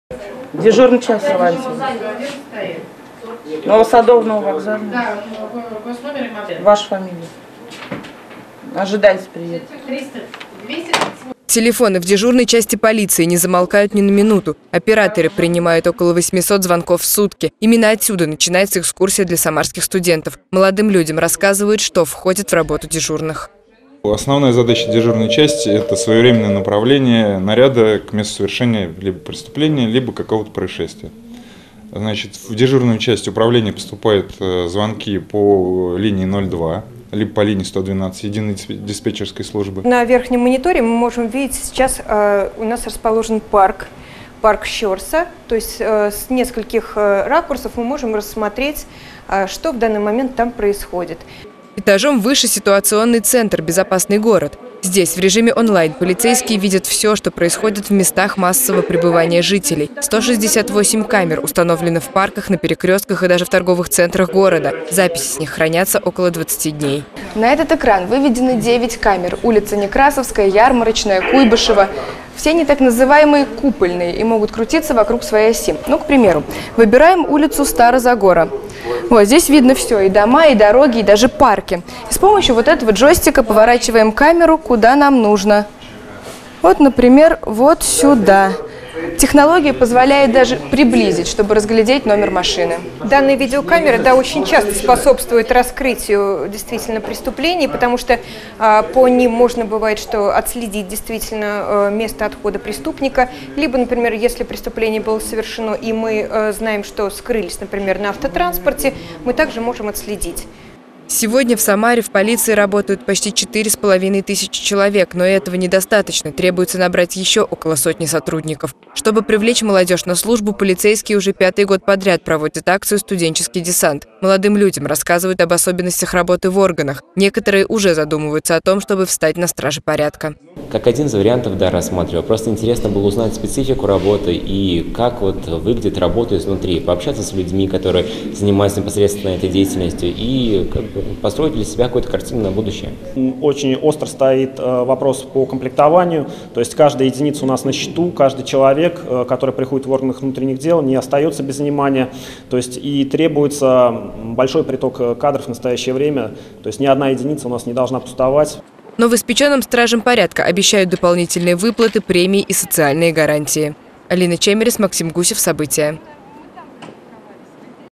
Студенческий десант в отделении полиции - стражи порядка провели экскурсию для самарской молодежи
Студенческий десант в отделении полиции - стражи порядка провели экскурсию для самарской молодежи. Показали, как оперативные дежурные следят за обстановкой в городе в режиме реального времени. Акция проводится уже пятый год подряд и направлена на привлечение студентов на службу в полицию.